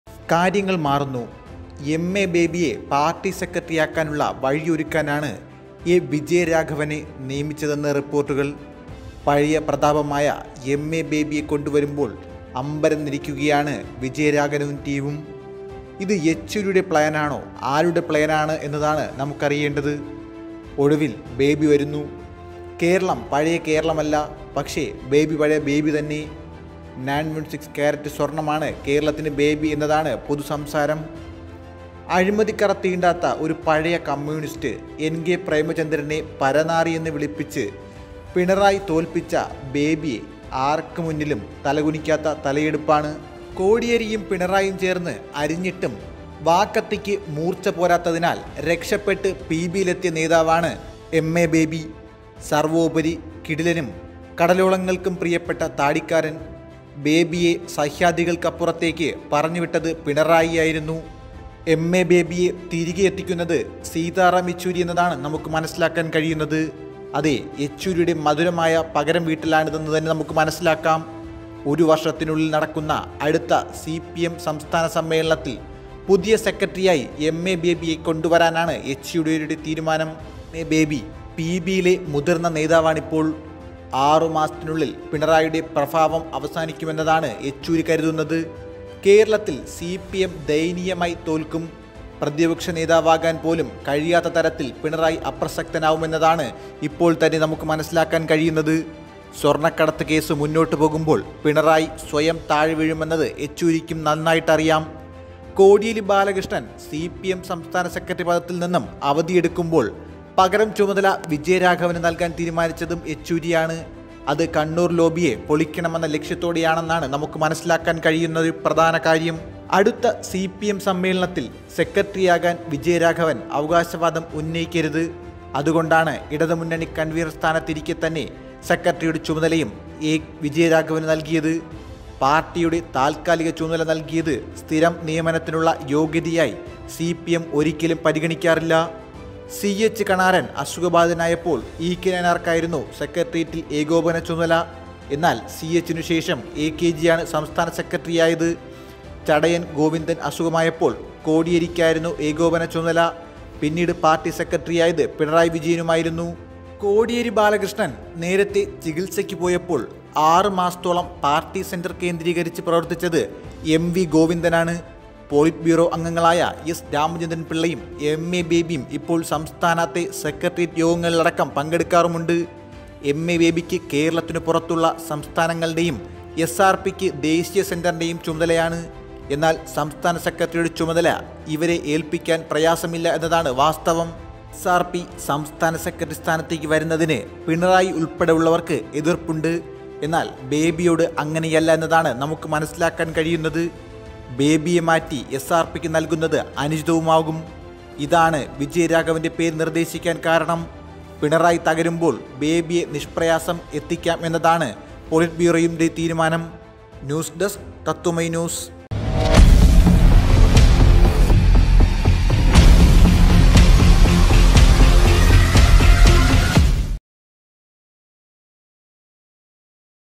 കാര്യങ്ങള്‍ മാറുന്നു.. എം.എ. ബേബിയെ കൊണ്ടുവരുന്നു..അമ്പരന്ന് എ വിജയരാഘവൻ! | CPM | KERALA
കാര്യങ്ങള്‍ മാറുന്നു.. എം.എ. ബേബിയെ കൊണ്ടുവരുന്നു..  അമ്പരന്ന് എ വിജയരാഘവൻ! ഇത് യച്ചൂരിയുടെ പ്ലാന്‍?